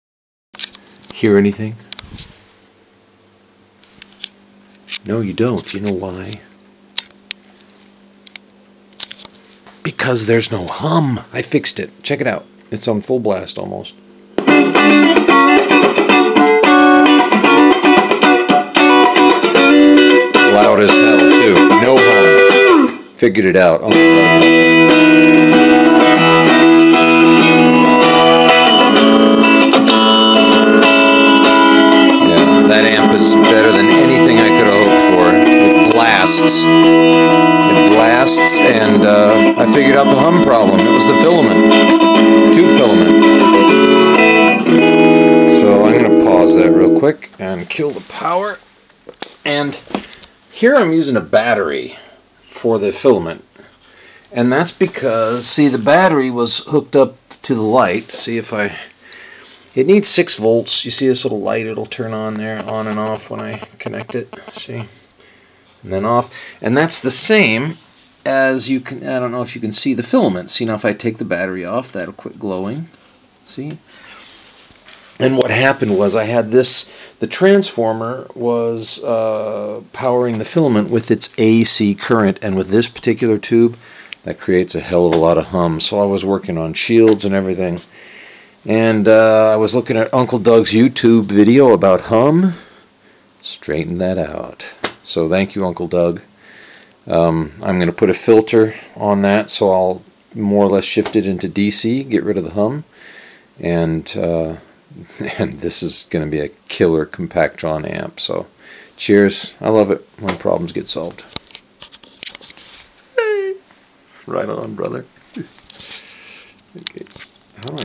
6AF11 Compactron Amp 2
Here I've done another test after trying to solve a problem with a 50 cycle hum. After a loud hum with no volume effect I tried isolating the heater filament with a 6 volt lead-acid battery and the hum totally went away...so it was only a matter of time to fix it.

12-pin B12C compactron sockets (where I got mine):

or just 2 pieces

The original idea and schematics comes from Forrest Cook.